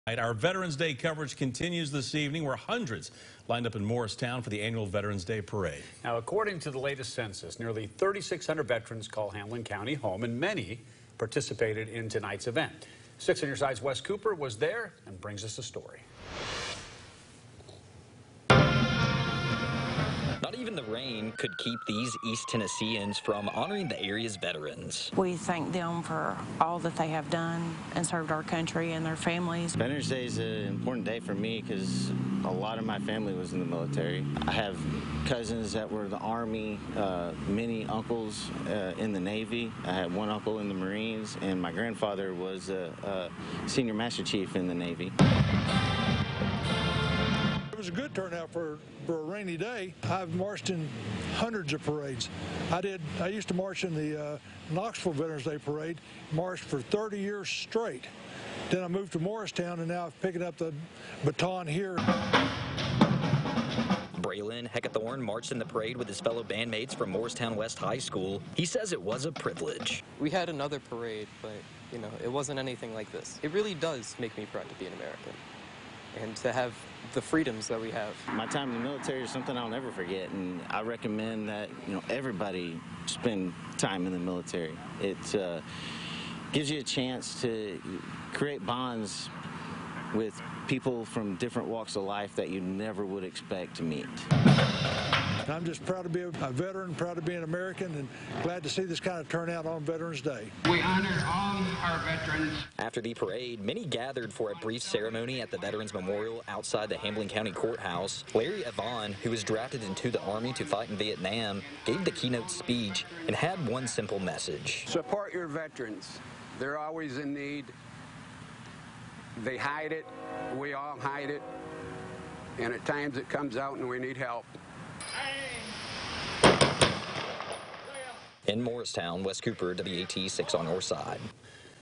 Veterans celebrated in Morristown with annual parade
Over 3,000 veterans call Hamblen County home and the rain didn't hold anyone back from celebrating them in Morristown Friday night.